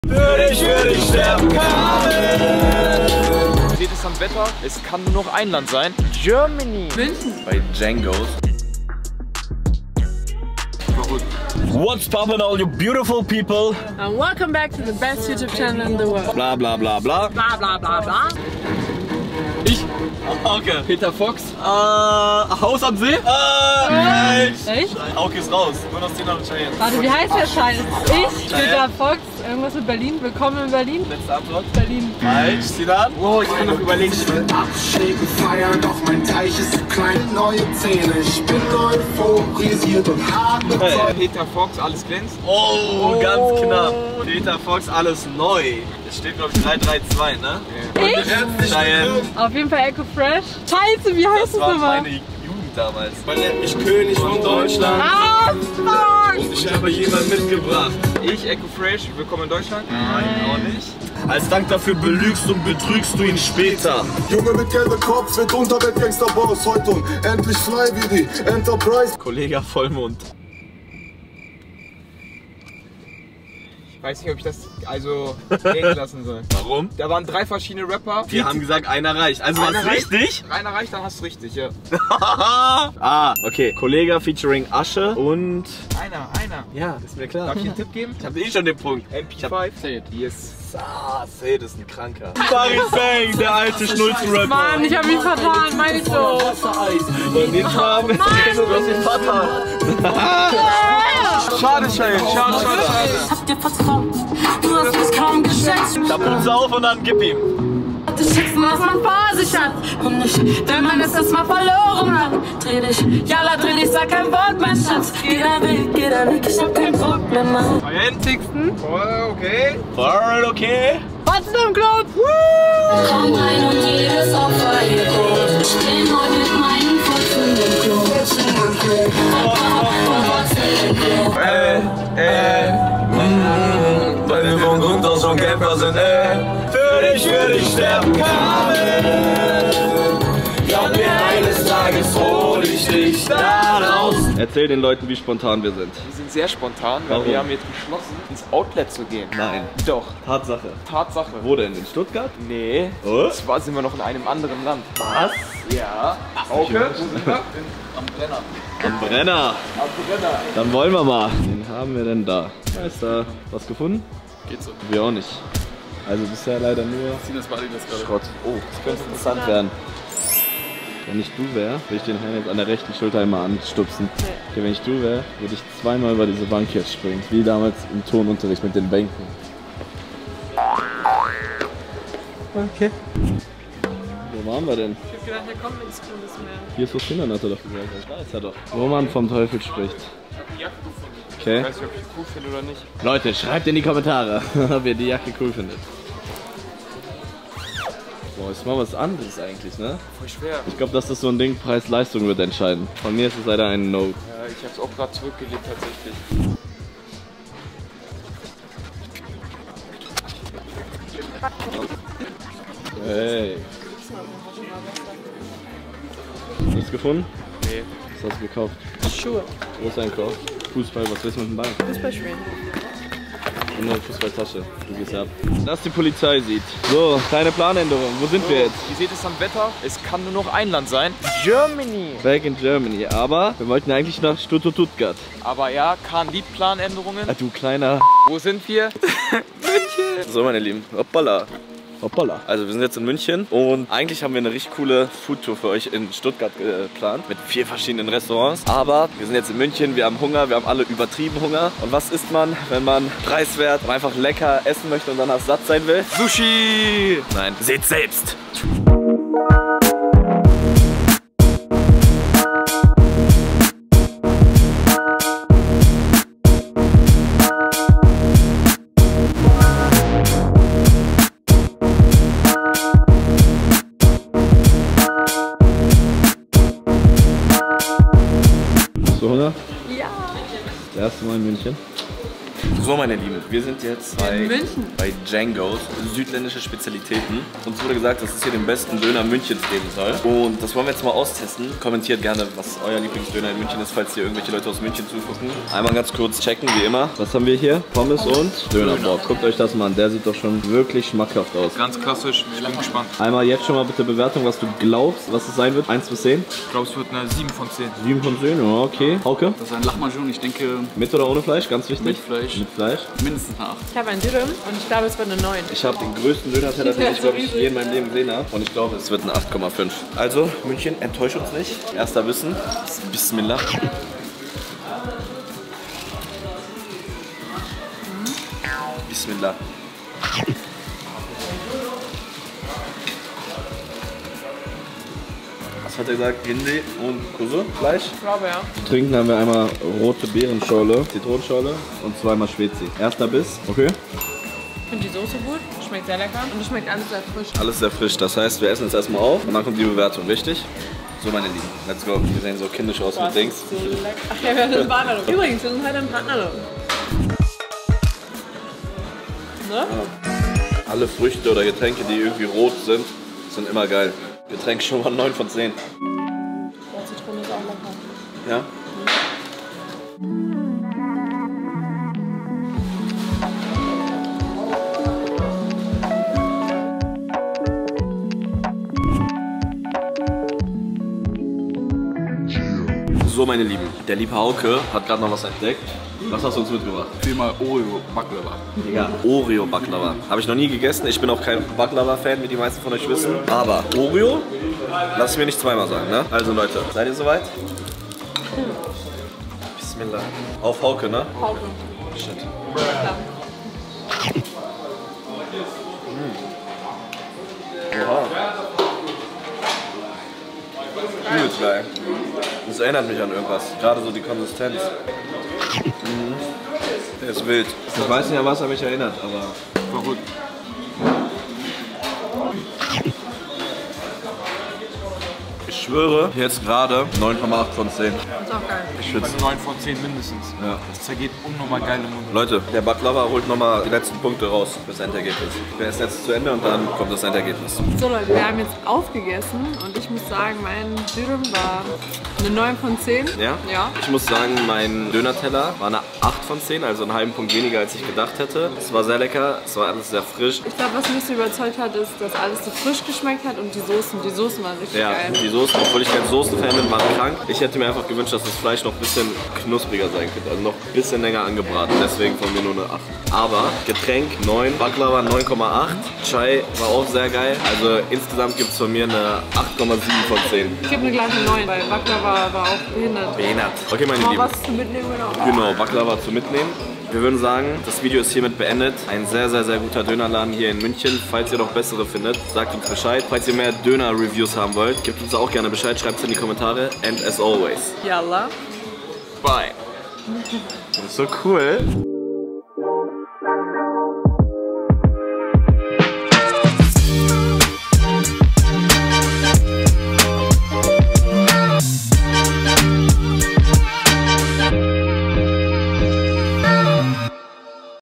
DJANGOS in Munich - The best KEBAB in GERMANY?
Hey,
wir hoffen, dass euch das Video gefallen hat.👍🏽
Dies ist die zwanzigste Folge von unserem neuen Format 'DJANGOS in Munich - The best KEBAP in GERMANY?'.🇩🇪
Wir sind auf der Suche nach dem besten Döner in ganz Deutschland. Falls ihr Empfehlungen habt, dann immer gerne her damit.
Unser erster Spot war Django's in München - die echt einen guten Start hingelegt haben.🥙
Schreibt uns gerne eure besten Dönerspots in die Kommentare oder bei Instagram.

Yallah, byeeeee!🤩

- CK🦋🐻